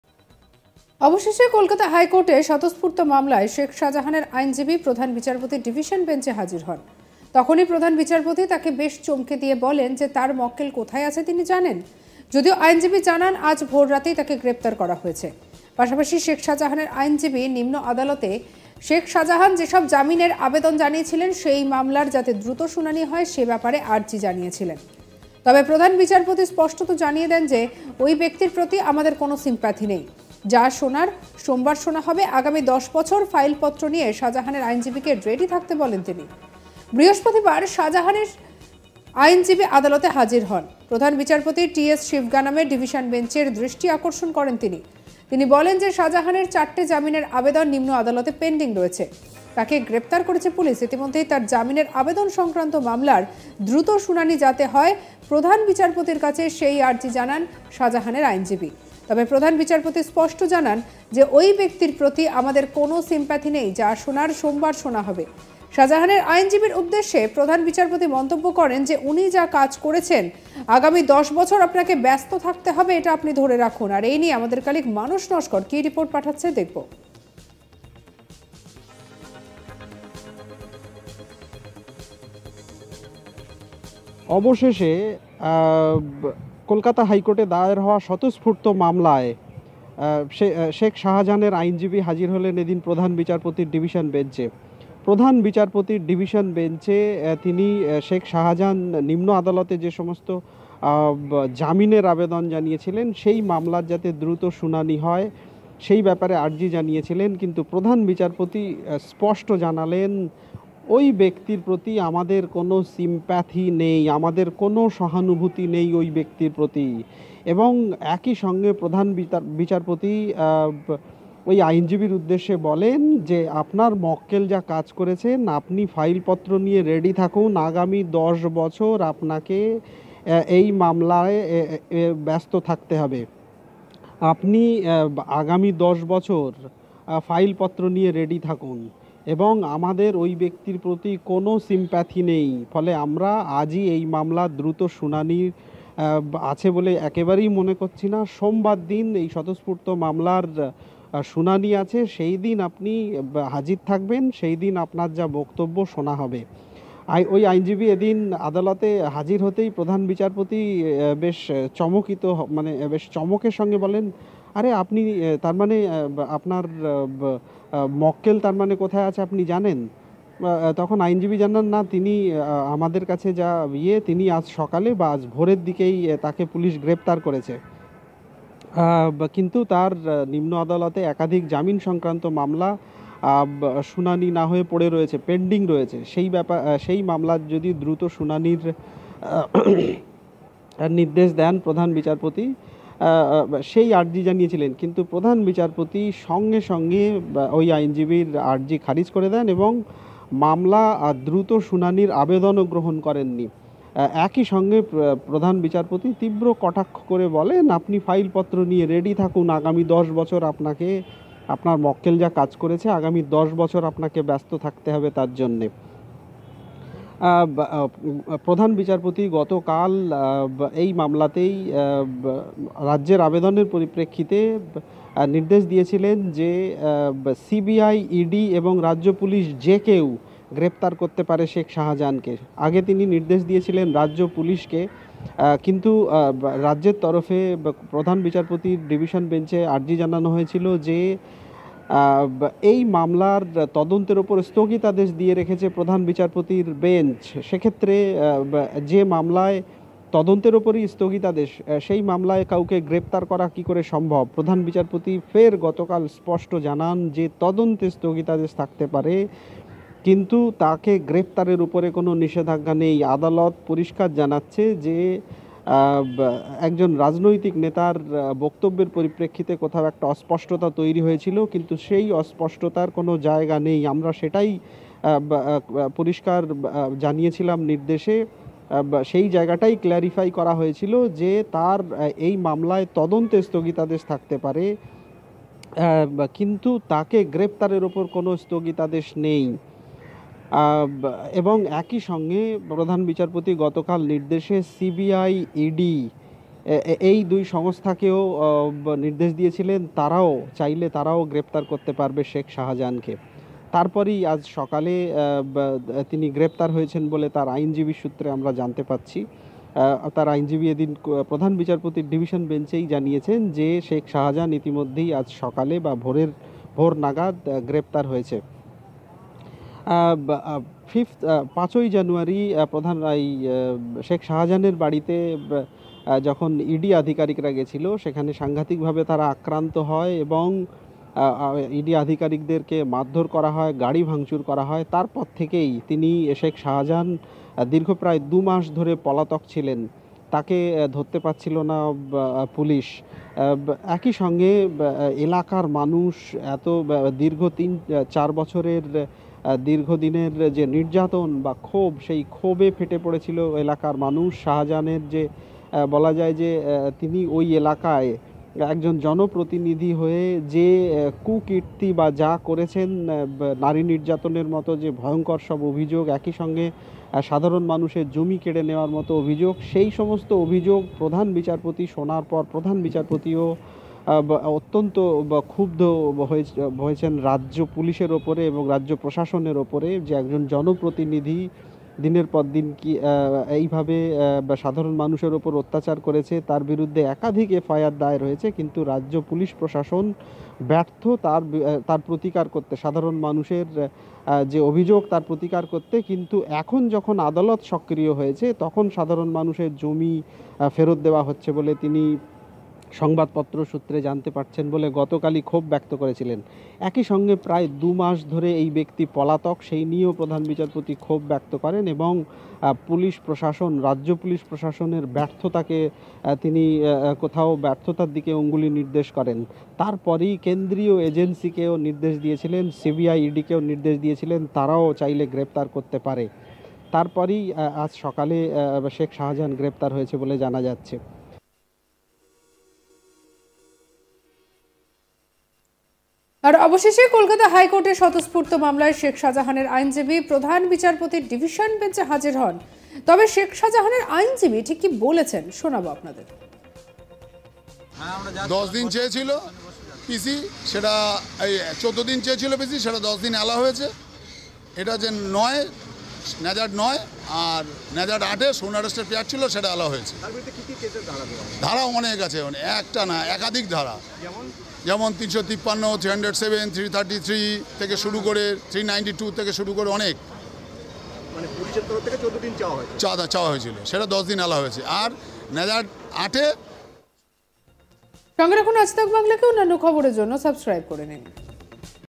Breaking 'আপনার জন্য অপেক্ষা করছিলাম' প্রধান বিচারপতি শাহজাহানকে খপ করে ধরলেন Sandeshkhali Shahjahan
অবশেষে কলকাতা হাই কোর্টে স্বতস্ফূর্ত মামলায় শেখ শাহজাহানের আইনজীবী প্রধান বিচারপতির ডিভিশন বেঞ্চে হাজির হন। তখনই প্রধান বিচারপতি তাঁকে বেশ চমকে গিয়েই বলেন তাঁর মকেল কোথায় আছে তিনি জানেন।  যদিও আইনজীবী জনান আজ ভোর রাতেইই তাঁকে গ্রেপ্তার করা হয়েছে।  পাশাপাশি শেখ শাহজাহানের আইনজীবী নিম্ন আদালতে শেখ শাহজাহান যেসব জামিনের আবেদন জানিয়েছিলেন সেই মামলার যাতে দ্রুত শুনানি হয় সেই ব্যাপারে আর্জি জানিয়েছিলেন।